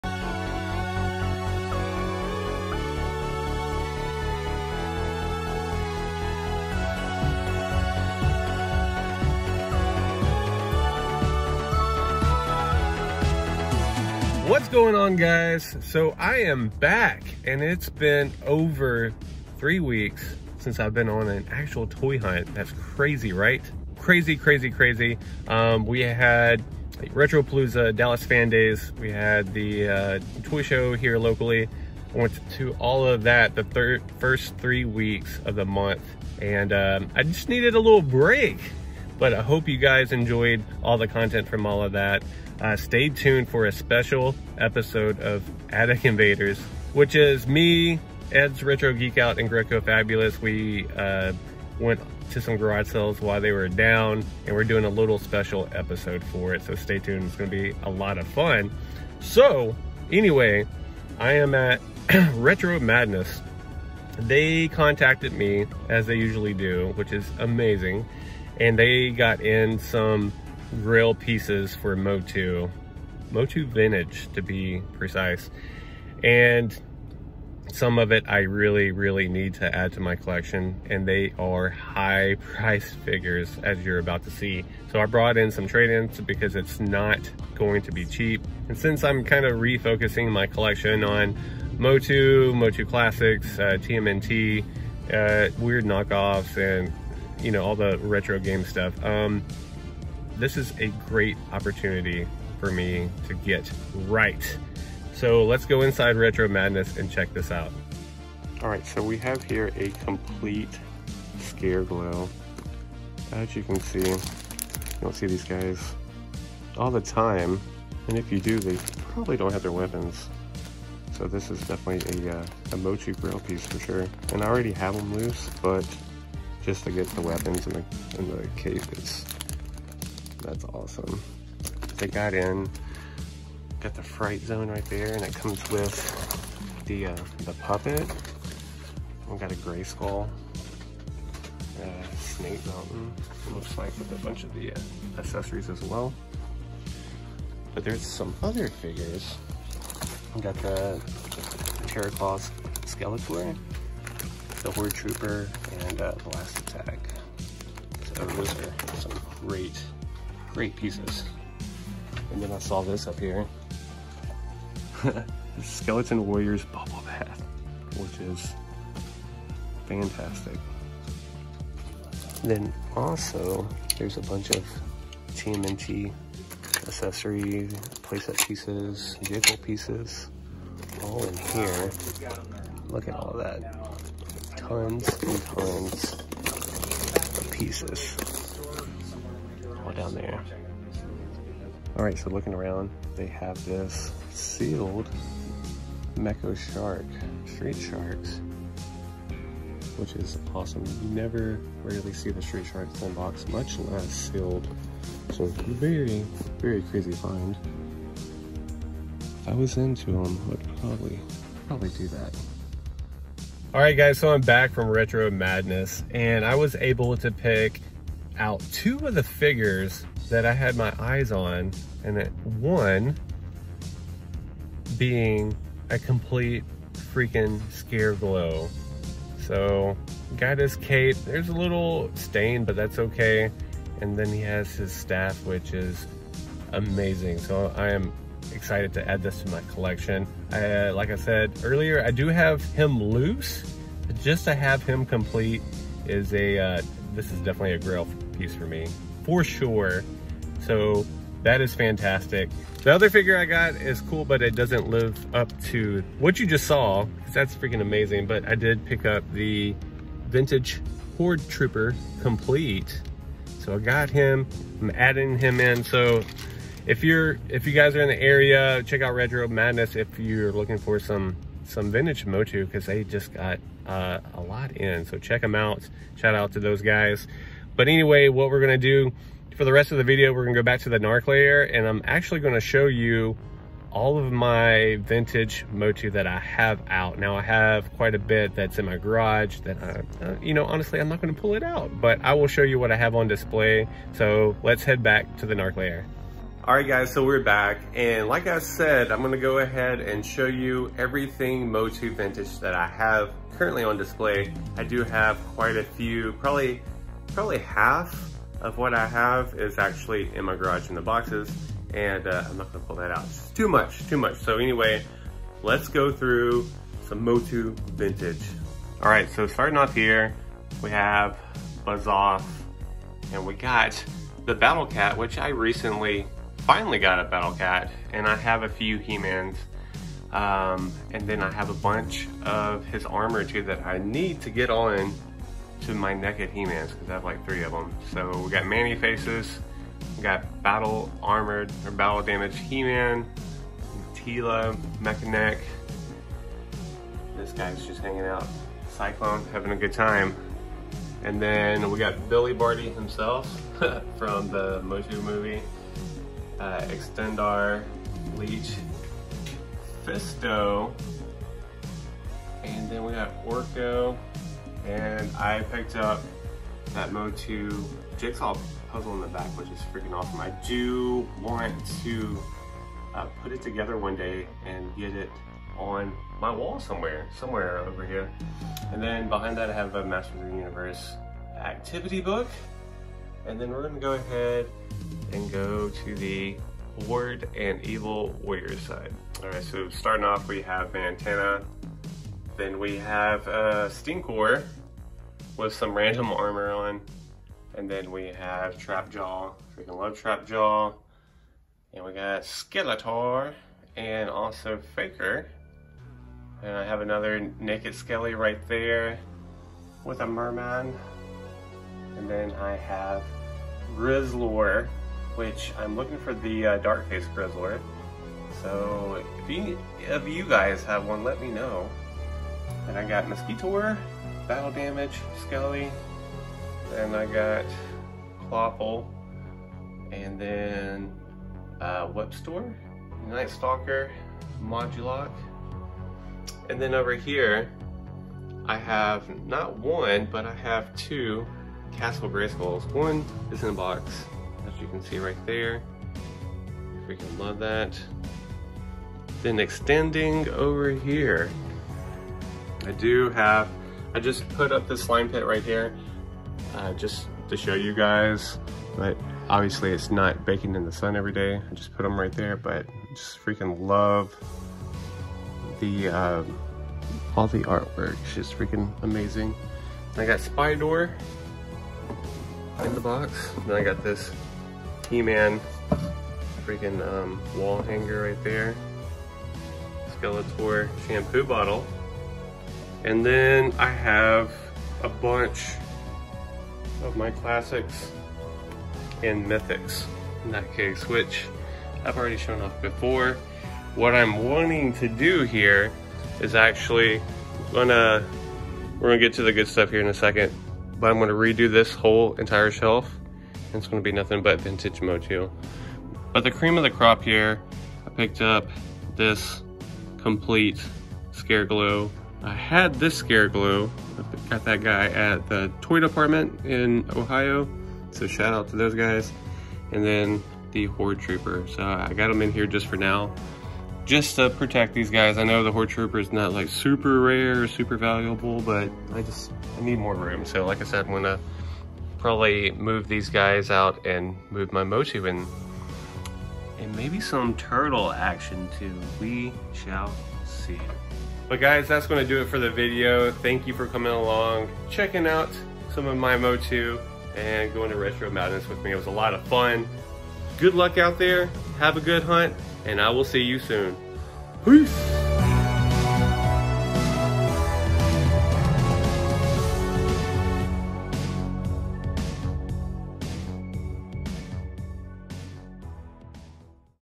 Toy Hunting MOTU GRAIL FOUND!
Join me as I pick up on of my grail pieces for the MOTU Vintage line and see some other awesome toy goodness on the way!